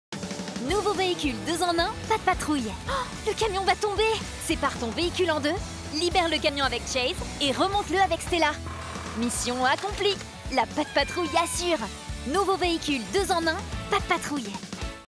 Paw Patrol - Véhicule 2 en 1 de Chase Pat' Patrouille
Un super véhicule inédit 2 en 1 aux couleurs de la Pat' Patrouille !
Appuie sur le bouton pour que le véhicule se déploie pour former deux véhicules et réussir la mission de sauvetage !
Un lance-projectiles à activer (2 projectiles inclus)
2 figurines fournies (Chase et Stella)
Existe également en modèle camion de pompiers, avec Marcus et Ruben

Pour plus d'information sur PAW Patrol: La Pat' Patrouille, visite notre site internet:

Retrouve les dernières nouvelles sur PAW Patrol: La Pat' Patrouille !